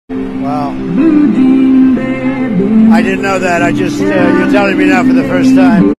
Explaining my Pug that Robbers usally don't knock
Explaining my dog that burglars usally don't knock at your door before robbing you, don't worry it is just dad coming home

Cute Pug confused bariking at knocks in house

Follow WhatDaDogDoin? for more doggo shorts 🌠